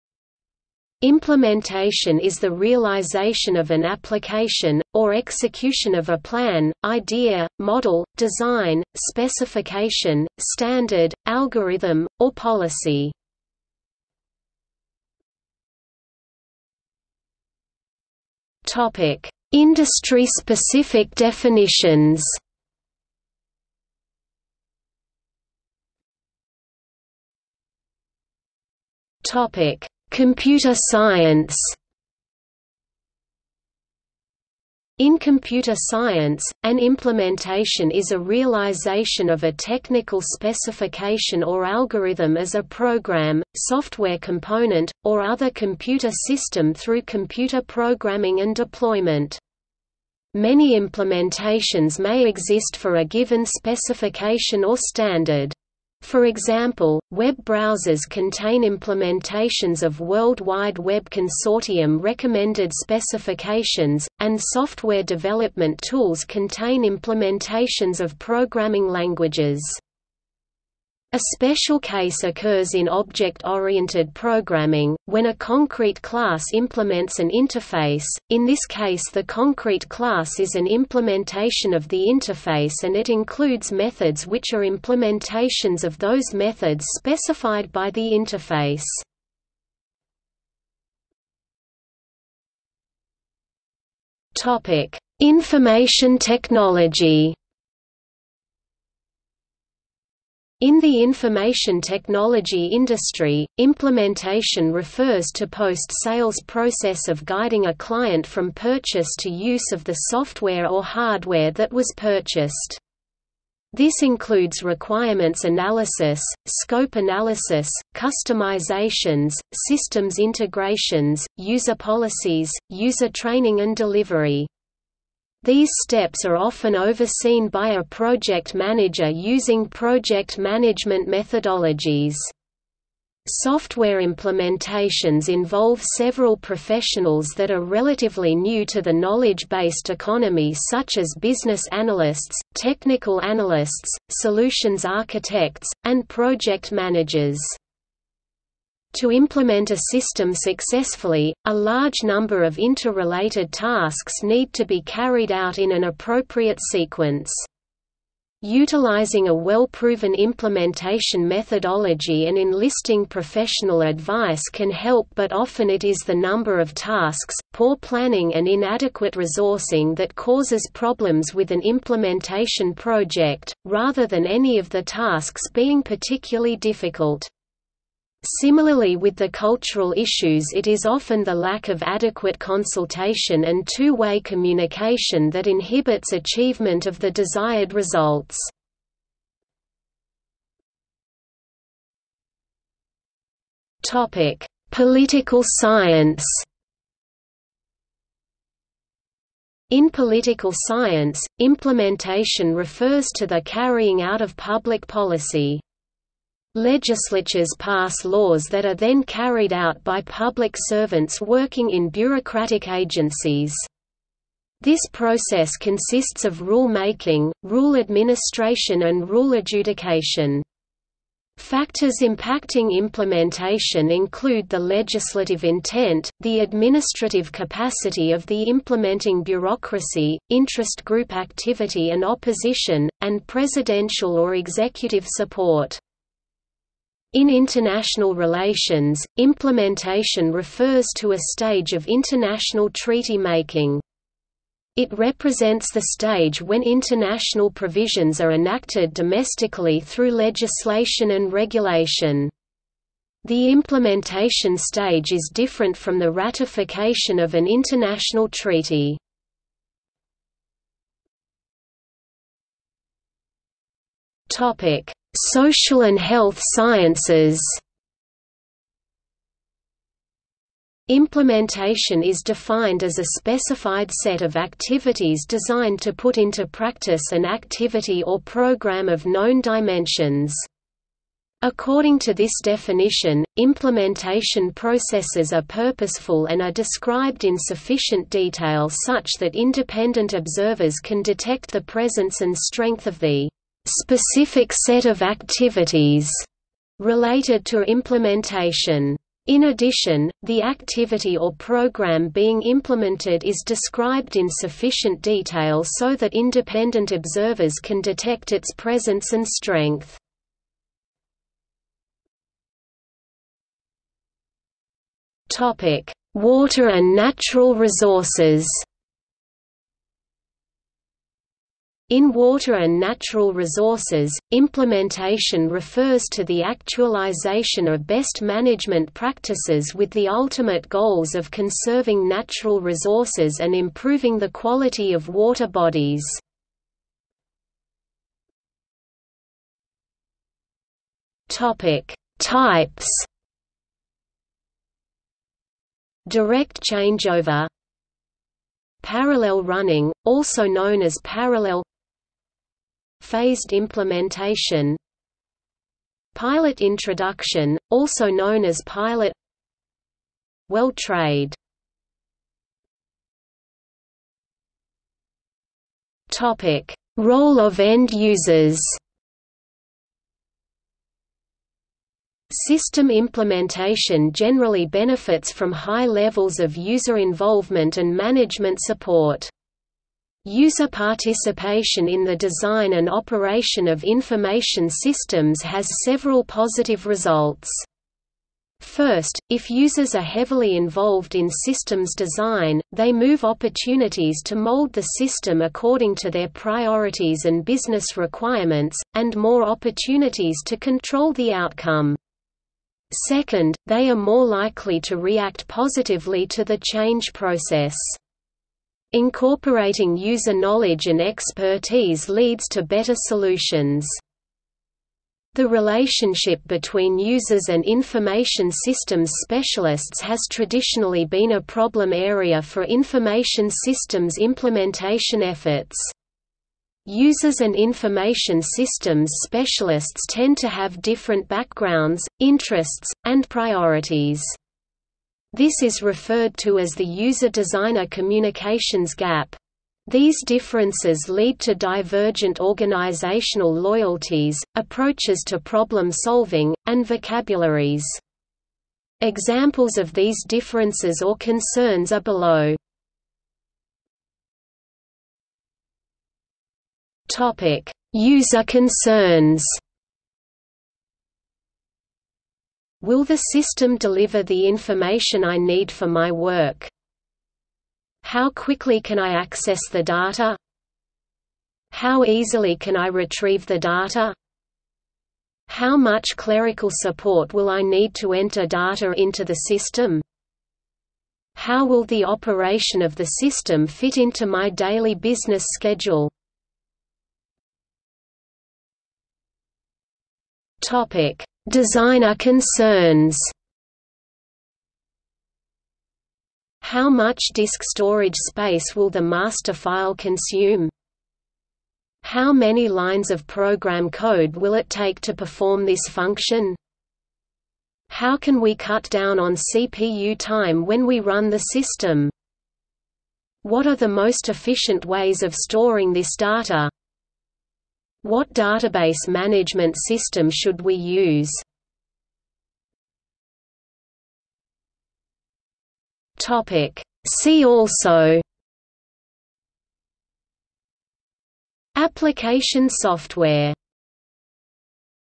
Implementation (computer science) | Wikipedia audio article
00:00:15 1 Industry-specific definitions
00:00:26 1.1 Computer science
00:01:31 1.2 Information technology
00:03:10 1.3 Political science
00:04:21 1.4 Social and health sciences
00:05:12 1.5 Water and natural resources
00:05:38 1.5.1 Types
00:06:02 2 Role of end users
00:07:33 2.1 User concerns
00:08:07 2.2 Designer concerns
00:08:42 3 See also

- Socrates

SUMMARY
Implementation is the realization of an application, or execution of a plan, idea, model, design, specification, standard, algorithm, or policy.